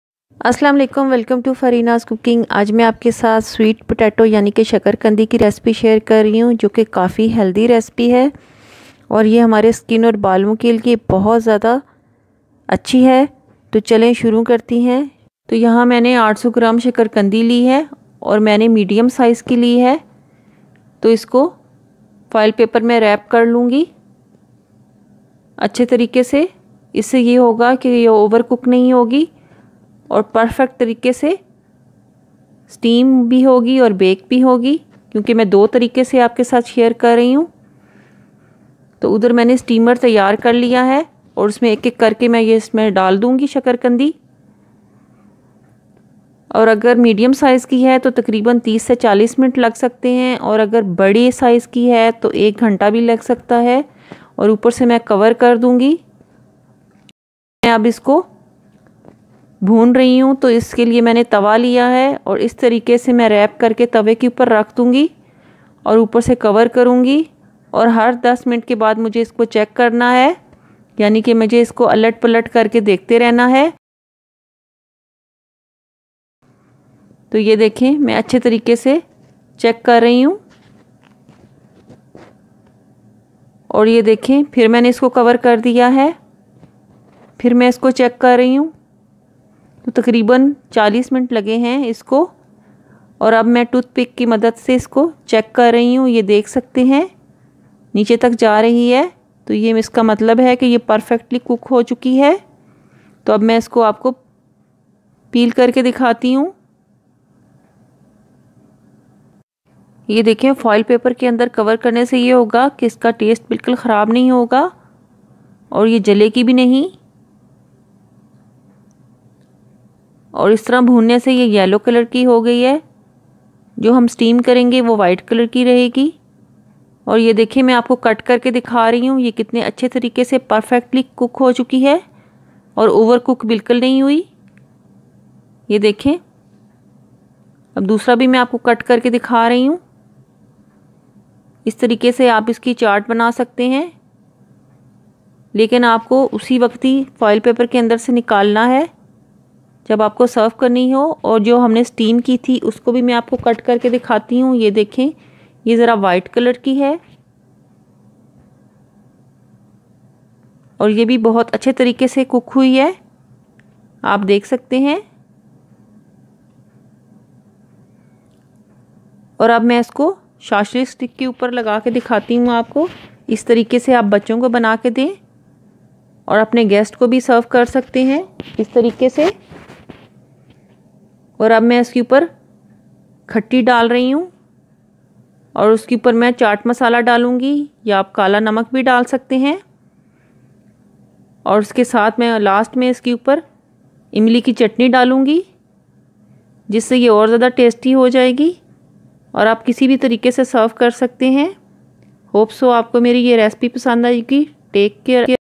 Sweet Potato Recipe 2 ways- Shakarkandi Recipe in Urdu
must try and share with family and friends.